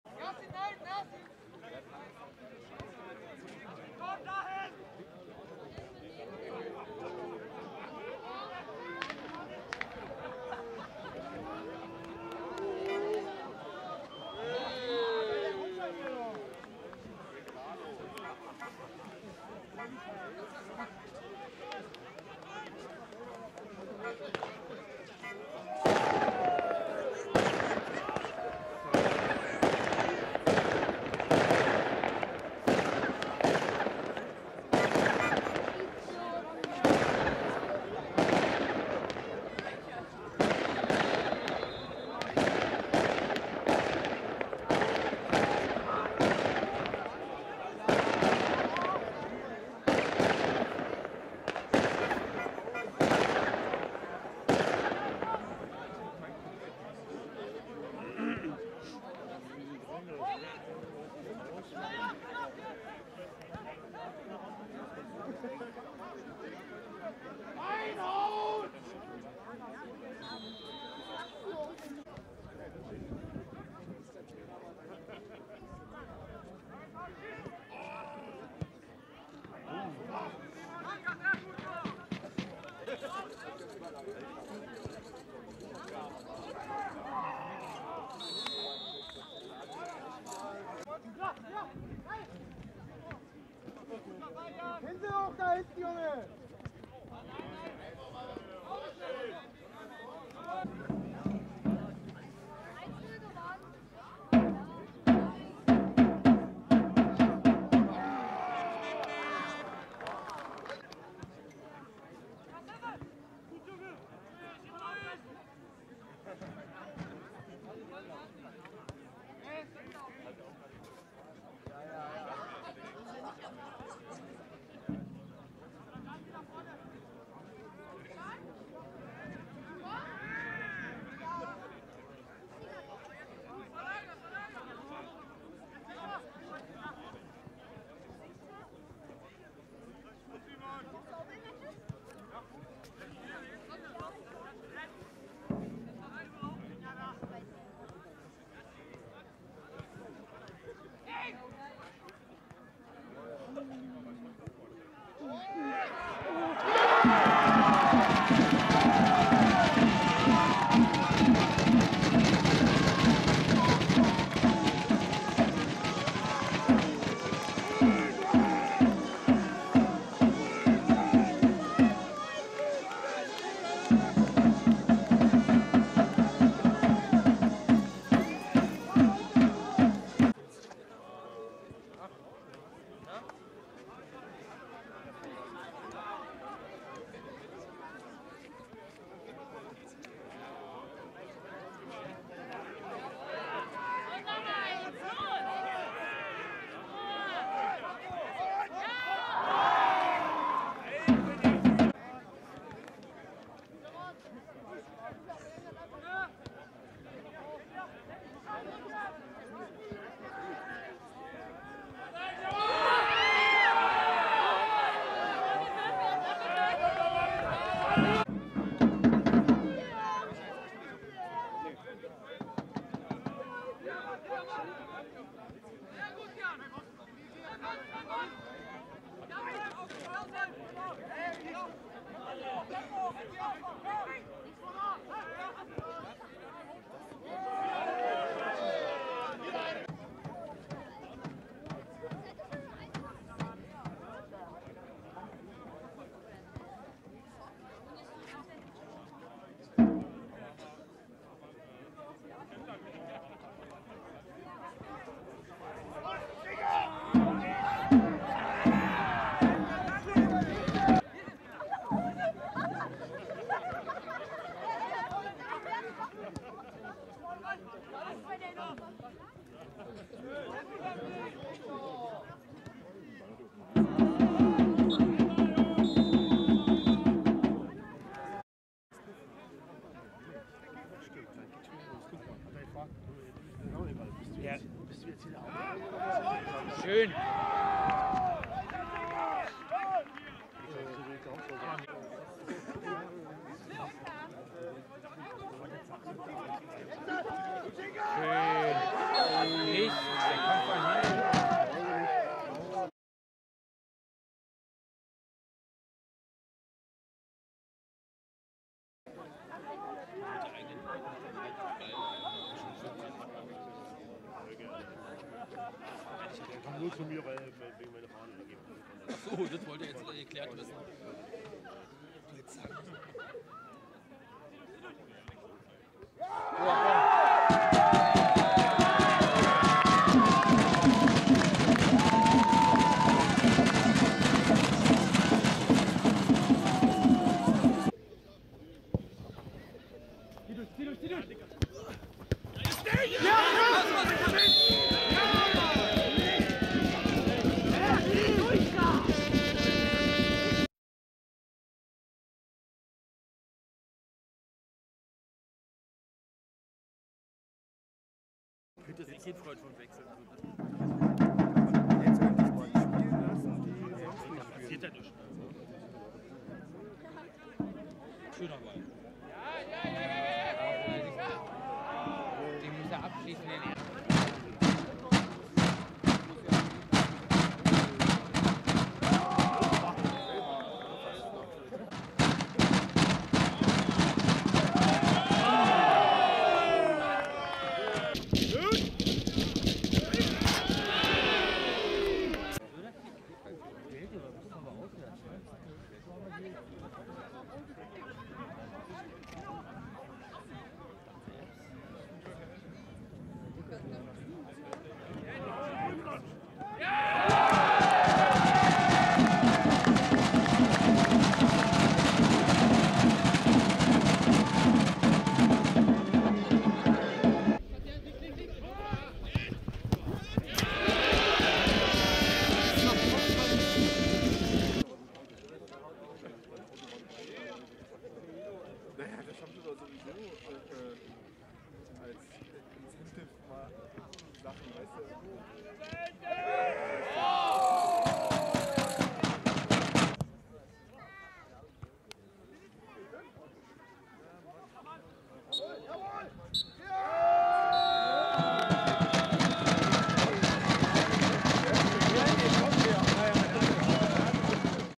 Brieselang - Falkensee 3:0 (12.3.2016)
19.Spieltag der Brandenburgliga
Grün-Weiß Brieselang gegen Falkensee/Finkenkrug

Tore:
1:50 1:0 Patrick Richter (31.)
5:14 2:0 Patrick Richter (52.)
6:39 3:0 Filip Marciniak (77.)

Kurz nach Anpfiff protestierten einige Fans von Falkensee mit Pyrotechnik gegen den Brieselanger Hauptsponsor (EDI.SON). Dabei blieb der Anhang vor dem Sportplatz und das Spiel wurde auch nicht unterbrochen.
Auf der Tapete stand: "GW Edison zerstört auch euern Sport", wenn ich das richtig erkannt habe.